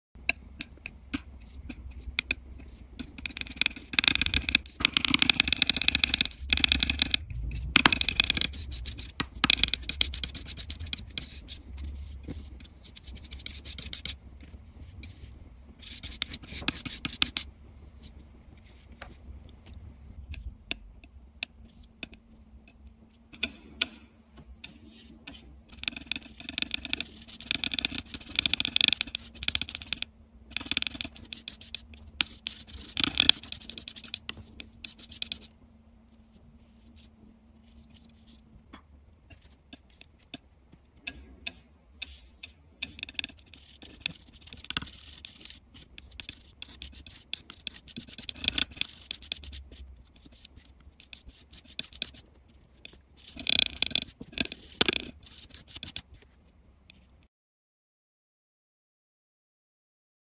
Who is your best? Which color did you like the most? Satisfying color mixing with tapping sounds
Satisfying color mixing with tapping sounds. Guess the mixed colors.